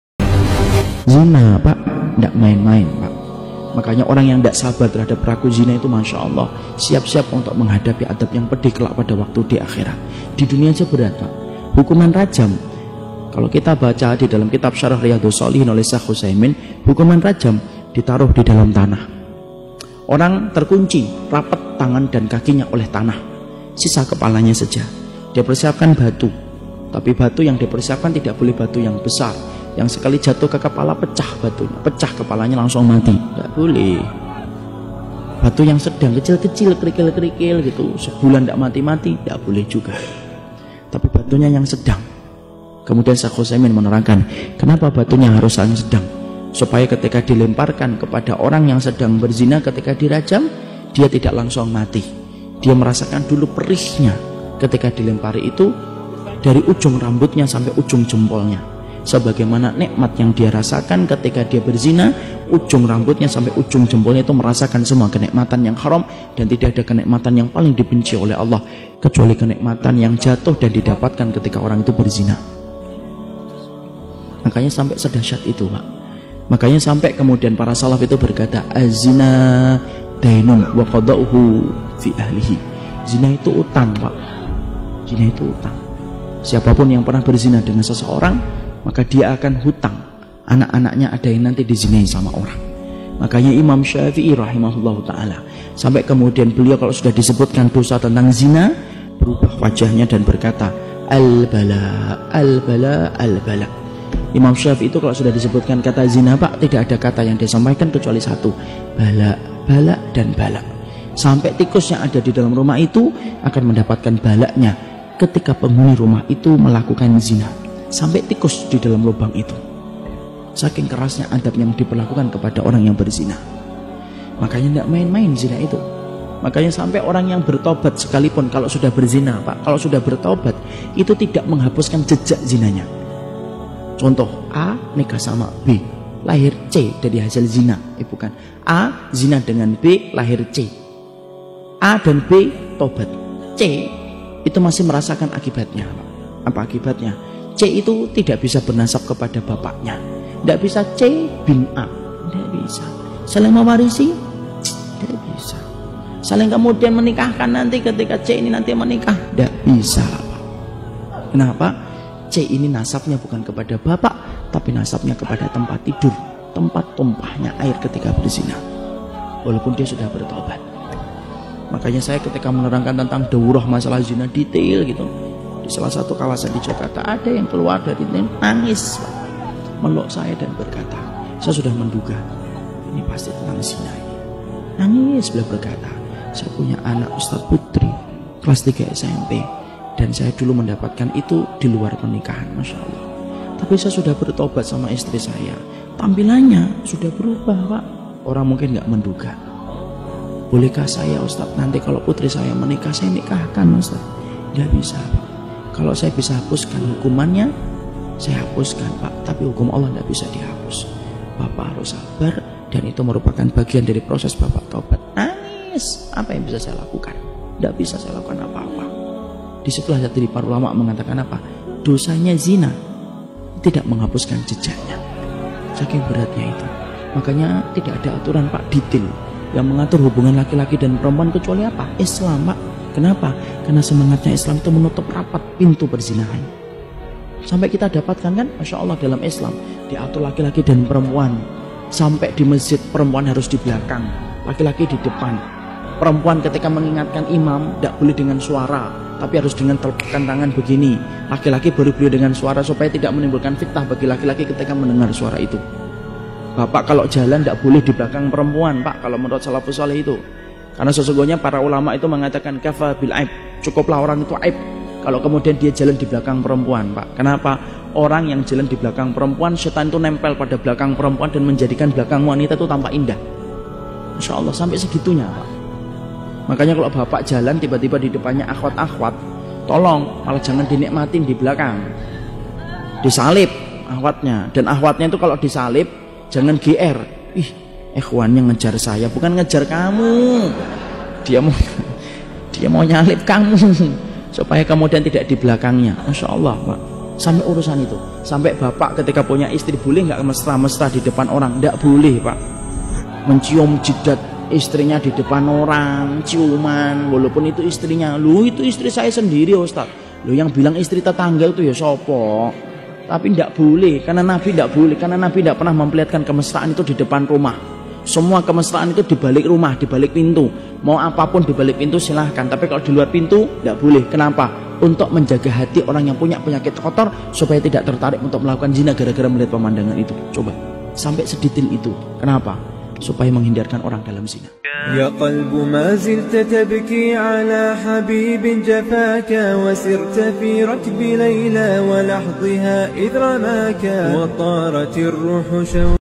Hukuman buat pezina
Hukuman bagi para pezina itu sangat keras